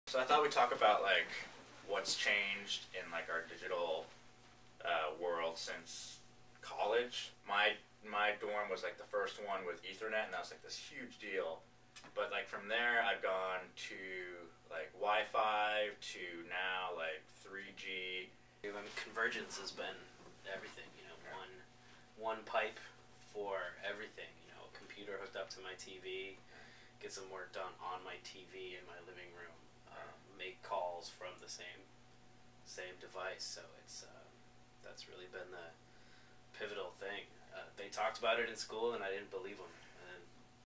Social Media, Convergence, and the Multi-Tasking Consumer
John Park and Andy Massouh, of Digital Influence Group, discuss the changing world of media, convergence, and the multi-tasking consumer.